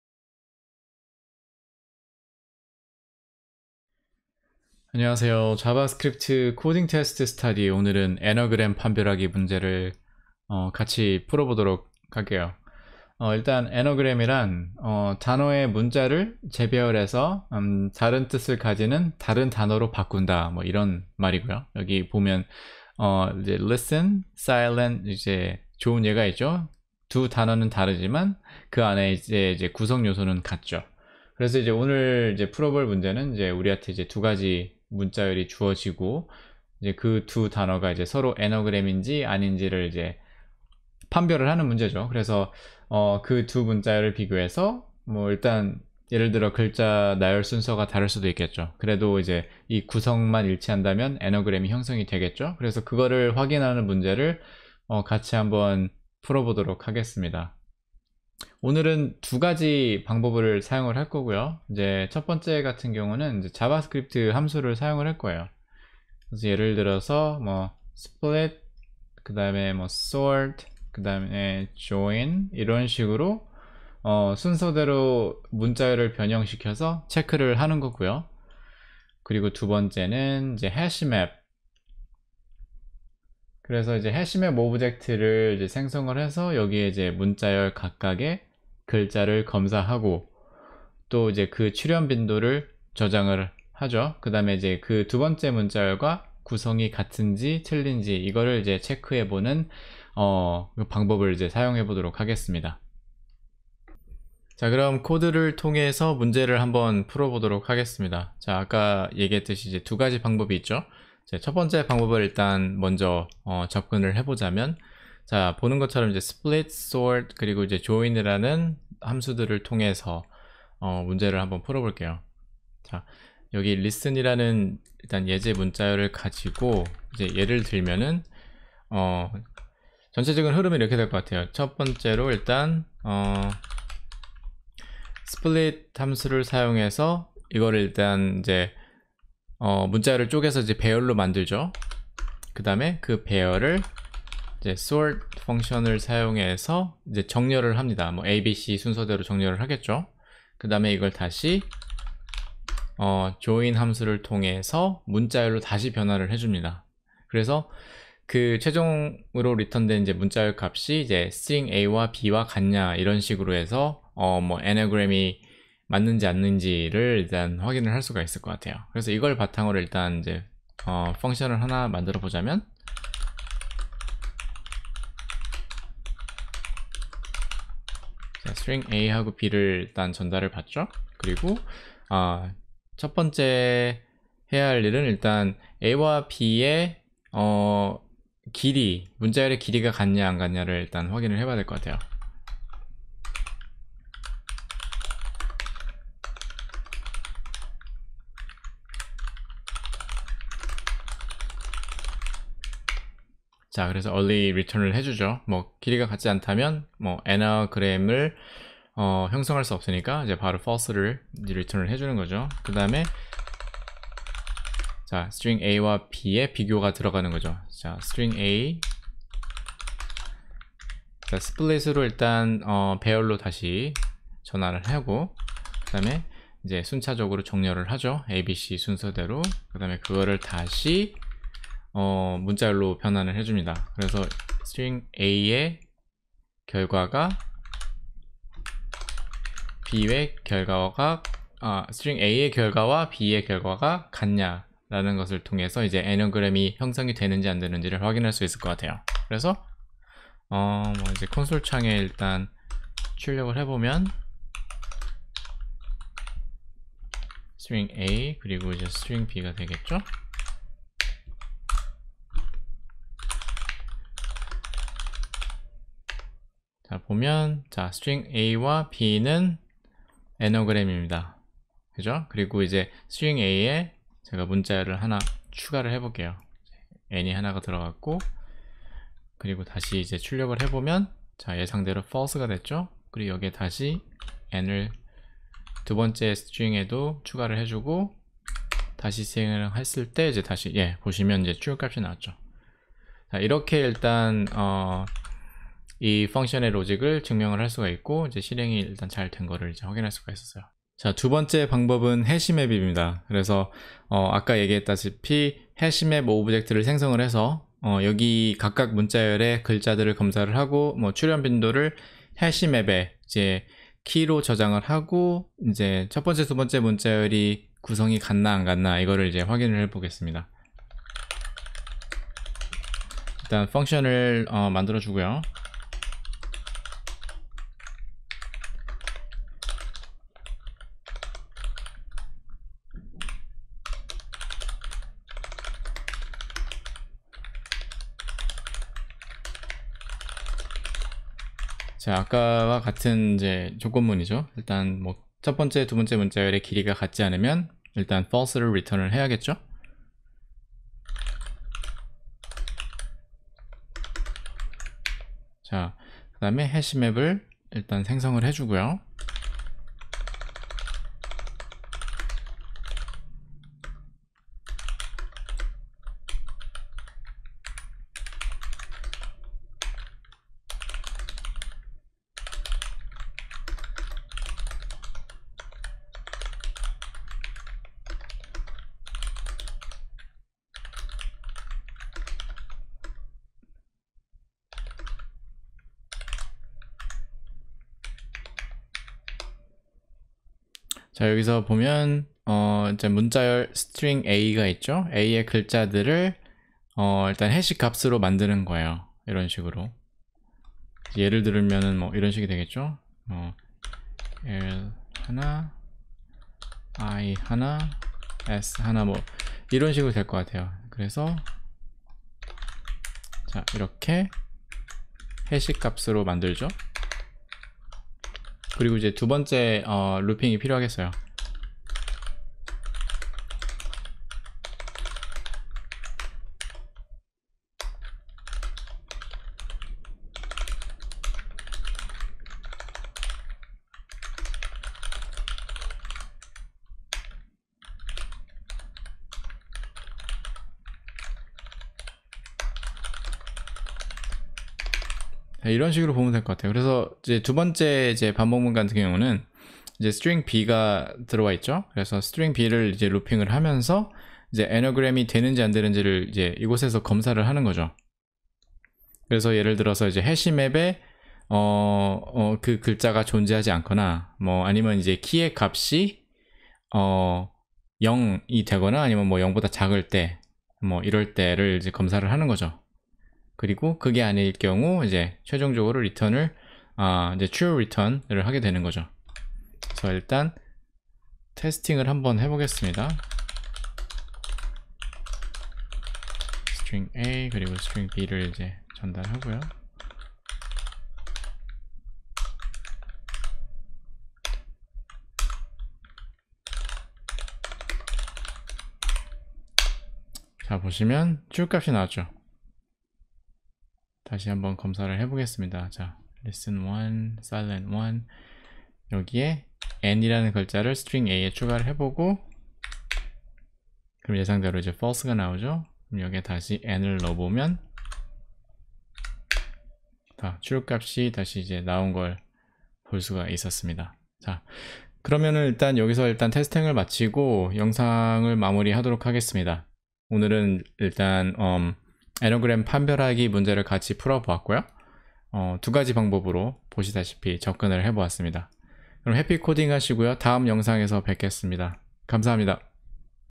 자바스크립트 코딩테스트 문제풀이 EP#2 | 애너그램 판별 (Valid Anagram)
자바스크립트 코딩테스트 스터디 영상입니다. 문자열의 애너그램 (Anagram) 판별 문제를 같이 풀어보도록 하겠습니다.

00:00 인트로
01:44 split & sort & join
05:00 hash map